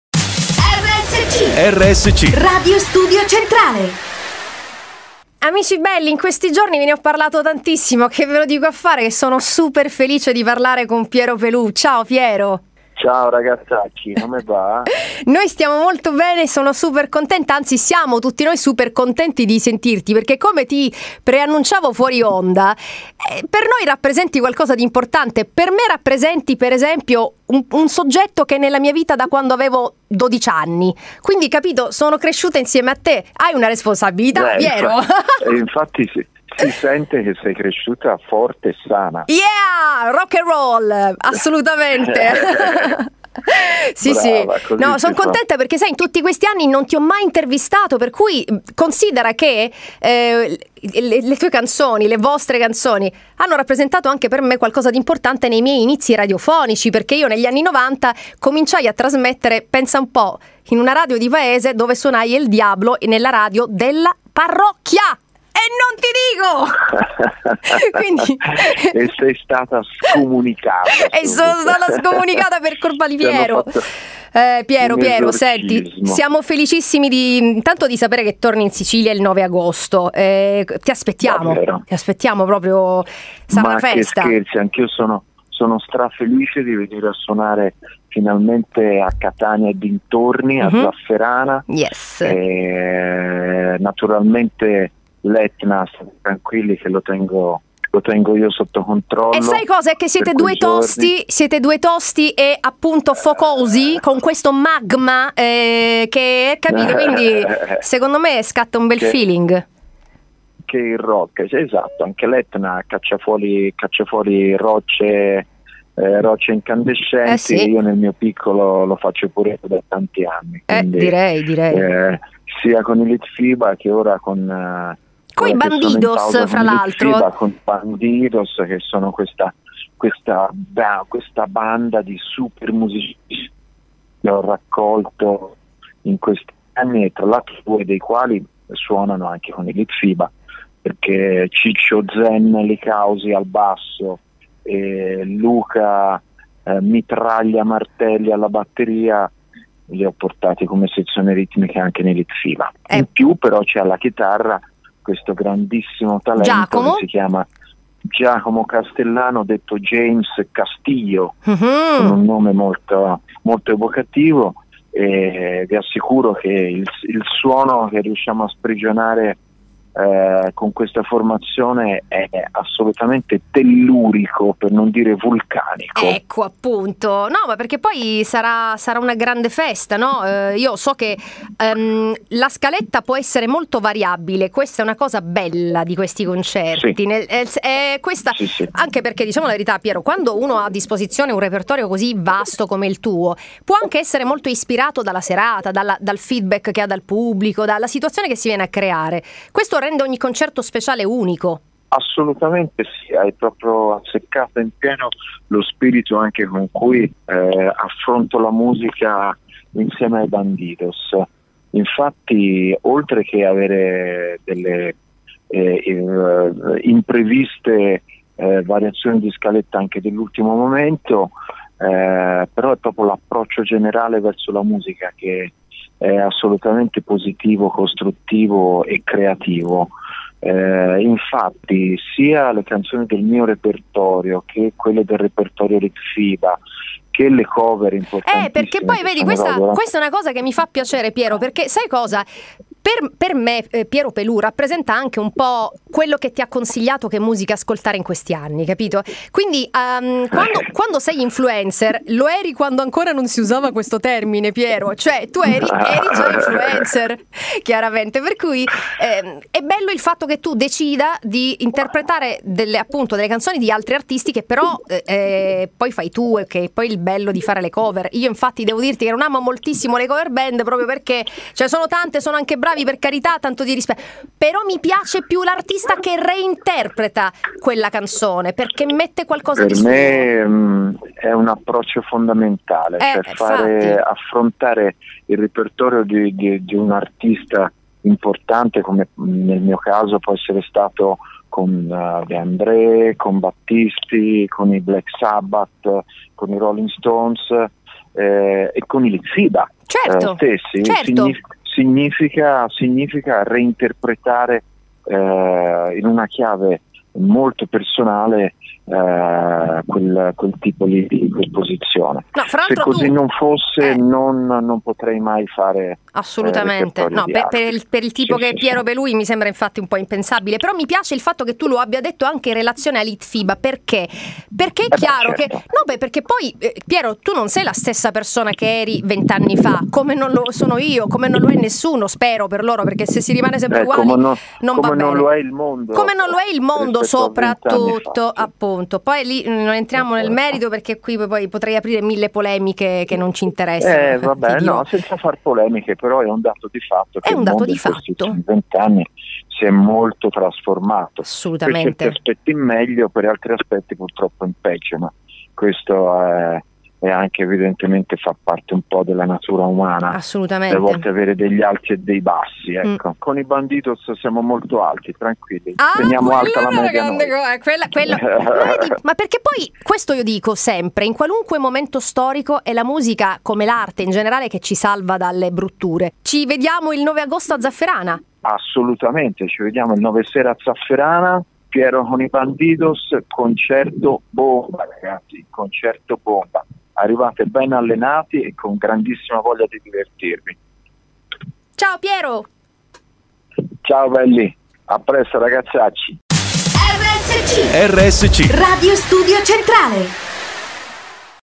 Piero Pelù 6.8.18
Piero Pelù si racconta in Salotto con la consueta energia, ospite di Antonella Insabella.